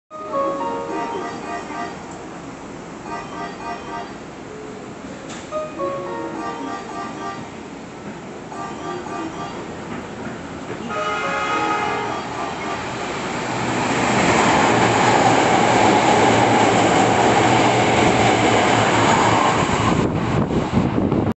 【12月で引退京都線の6300系運用が見納めに】6354F 快速特急A 京都河原町行き
撮影日不明

ダイヤ改正で新種別｢準特急｣が関西で復活ですね‼️